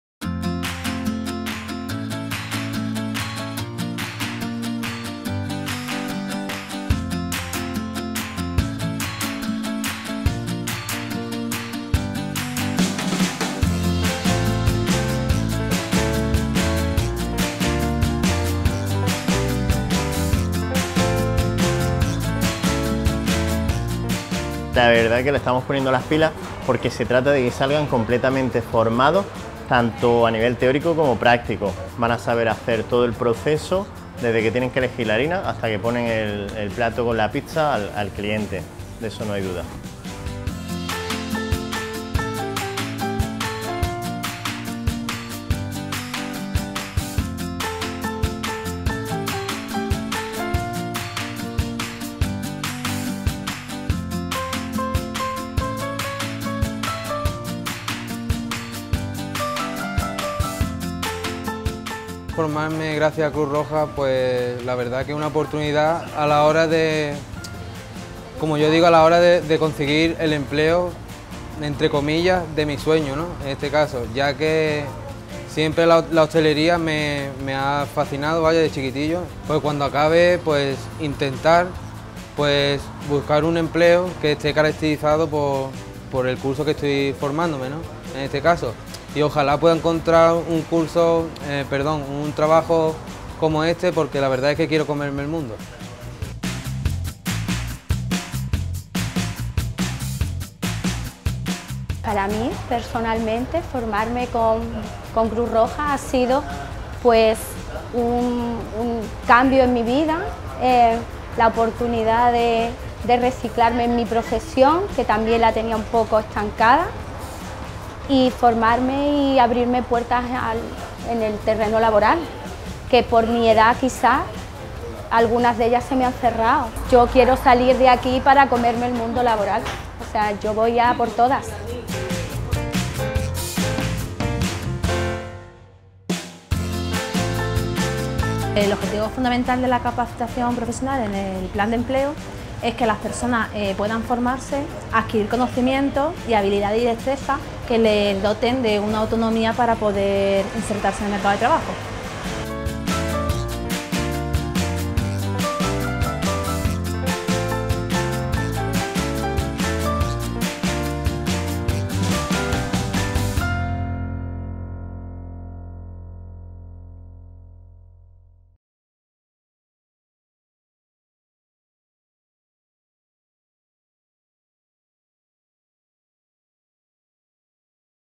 Entrar en harina para comerse el mundo
El gerente del restaurante L´Albero, tricampeón del mundo en pizza acrobática, se suma al reto social empresarial de Cruz Roja en Málaga. Desde la elaboración de la masa, hasta el emplatado de la pizza, nos enseña a entrar en harina para comernos el mundo.